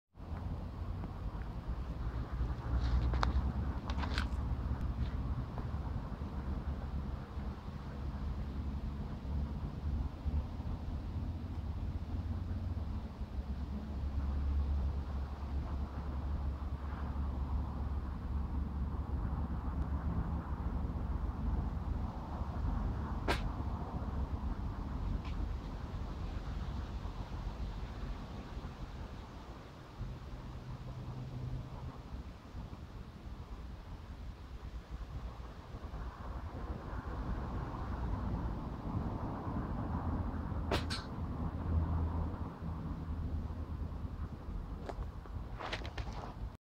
shooting 30 yards with smk qb78 co2 air rifle
Just taking test shot after lining scopes up by shooting a can off a tree